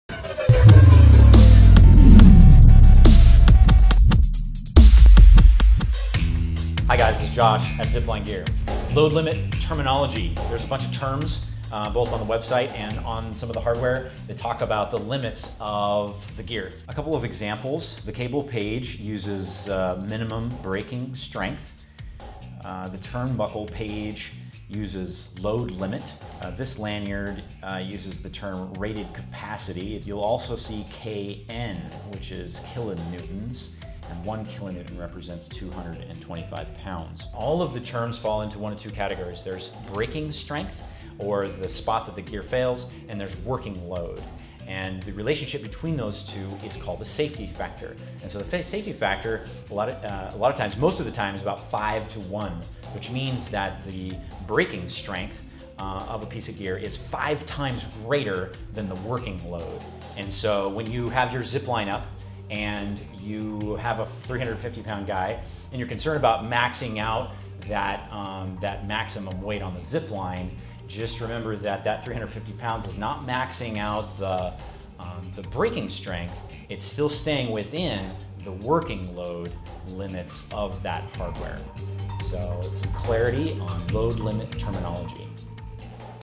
Load Limit Terminology
When discussing the load limits of gear there are a number of terms that get used.  All these terms can be put into one of two areas, the working load and the breaking strength.